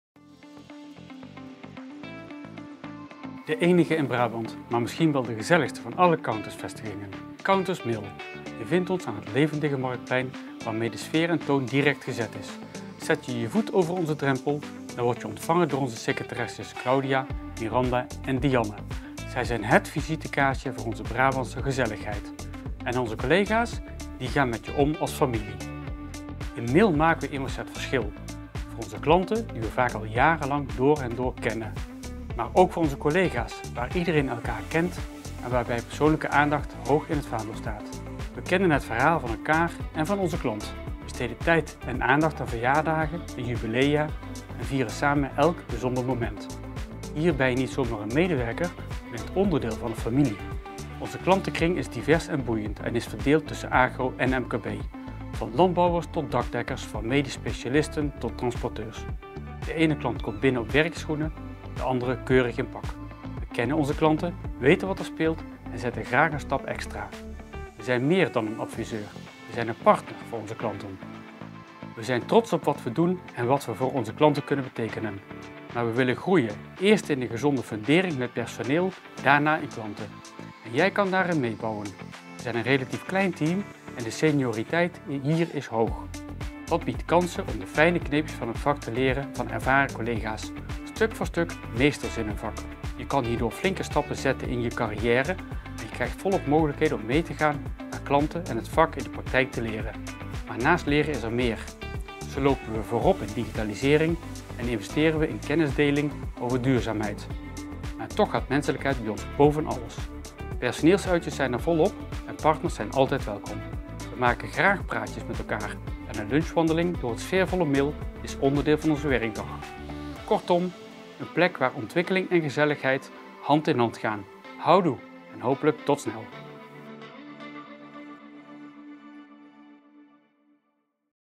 Countus Mill
We hebben slechts één locatie in Brabant, maar dat is gelijk wel een heel fijne. Countus Mill: een plek waar gezelligheid en expertise in het vak hand in hand gaan. Er is enorm veel kennis aanwezig in ons team, en daar kan de jongere garde in het vak de vruchten van plukken. Flinke stappen in je carrière zijn hier dan ook zo gezet.